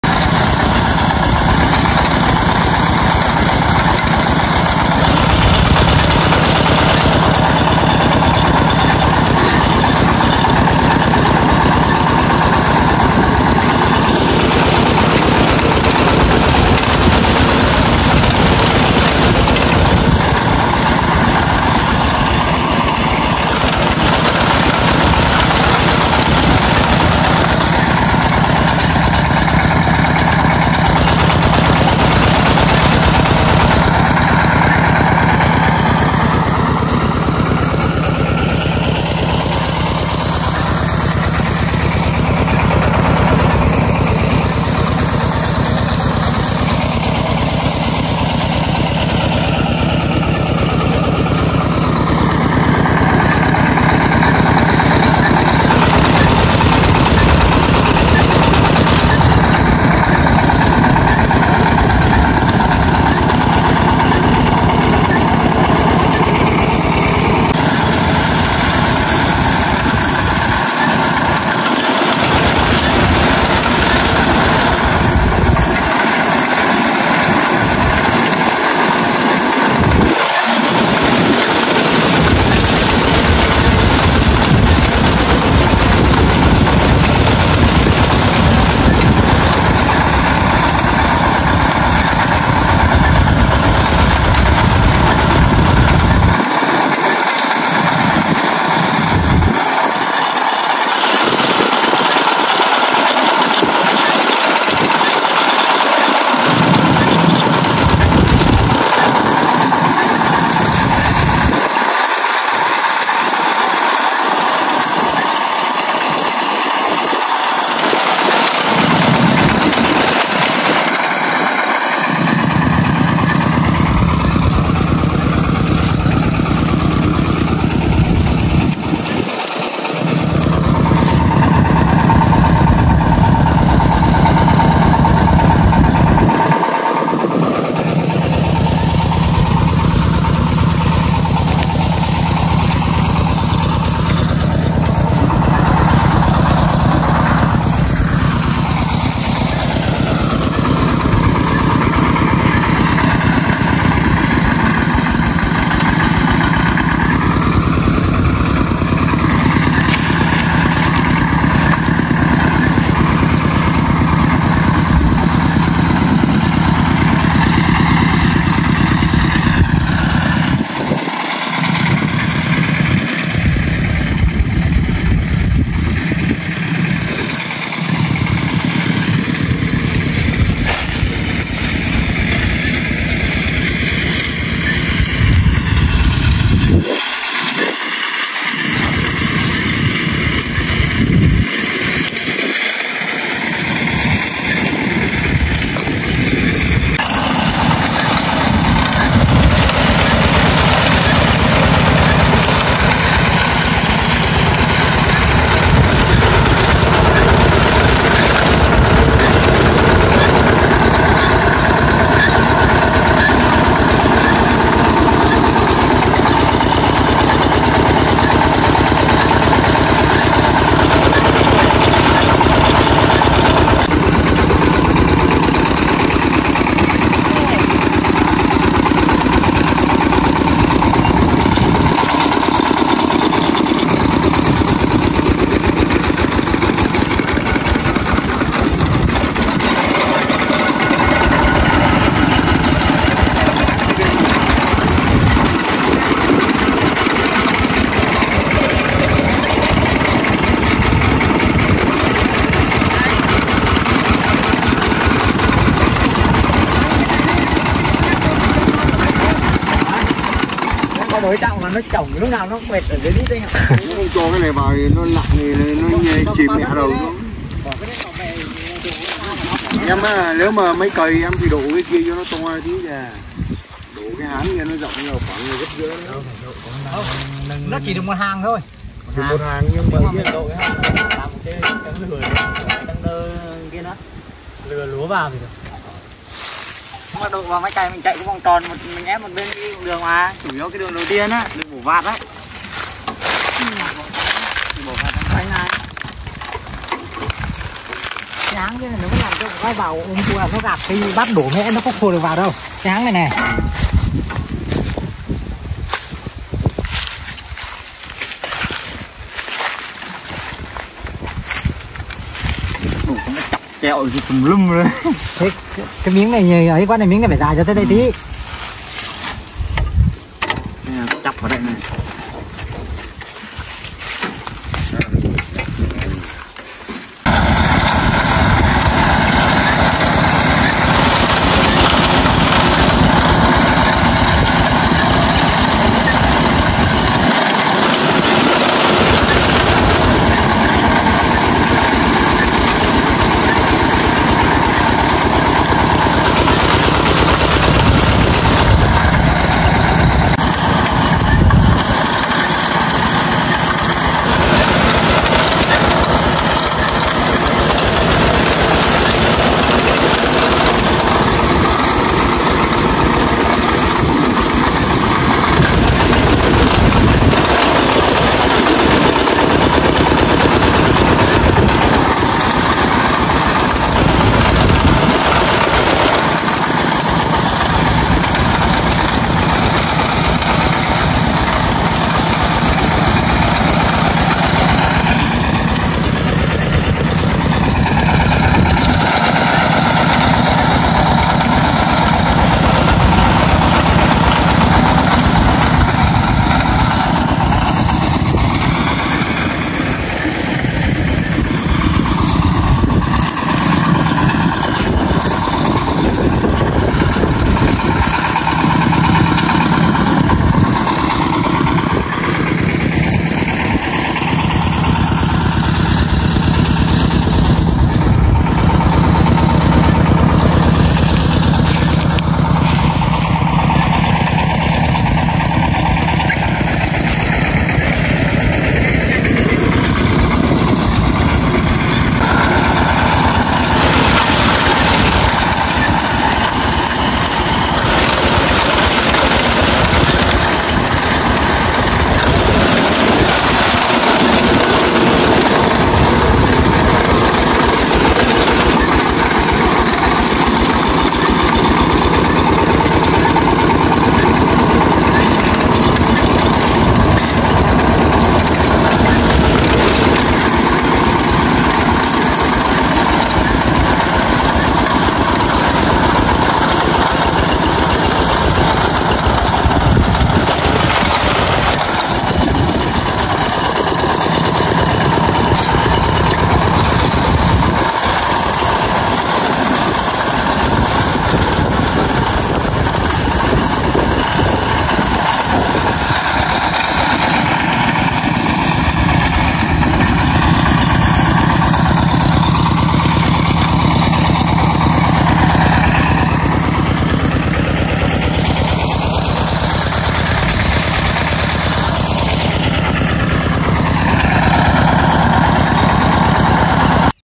Máy bẻ bắp kết hợp băm cây gắn vào đầu công nông ở Eakar, Đăk lăk.
Video này mình sẽ chia sẻ về máy bẻ bắp kết hợp băm cây gắn vào đầu công nông ở eakar, đăk lăk. Mọi người cùng đón xem ạ!